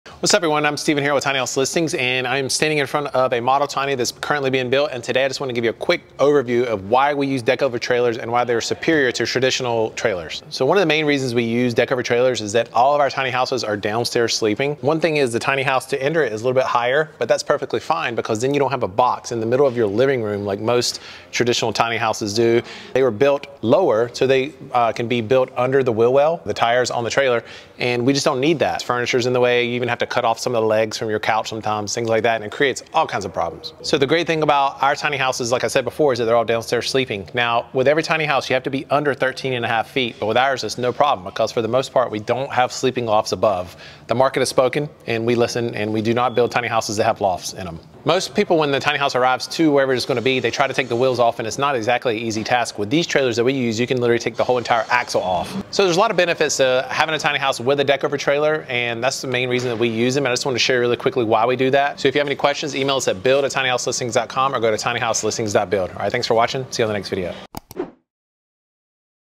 Why We Love Deck-over Trailers
Discover the secret behind our sturdy tiny homes! 🏡💪 We opt for deck-over trailers, ensuring every inch of the floor is flat and stable. Plus, our trailers feature removable axles, making transport a breeze. 🛞Quality craftsmanship meets practical design. Ready to experience the difference? 💻